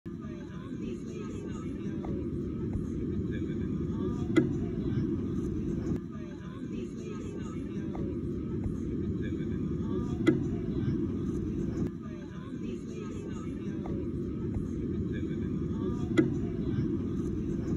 A two-headed seaturtle just hatched in myrtlebeach
A newborn two-headed loggerhead sea turtle was released into the Atlantic Ocean in Garden City, just south of Myrtle Beach, southcarolina after local enthusiasts discovered it.

Video by: Terry Ann Graham a volunteer with South Carolina United Turtle Enthusiasts